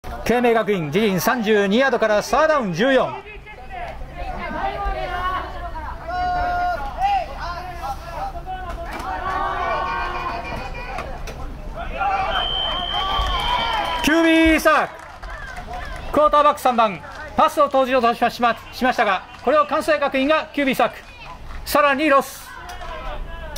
20210718 啓明学院中学校vs関西学院中学部 0216 Kwansei DE77 QBSuck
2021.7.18 第19回関西中学アメリカンタッチフットボール選手権 西地区
啓明学院中学校×関西学院中学部
【Pick-Up Play】

※試合のキープレーをピックアップしています。
※チーム校要請による撮影配信になります。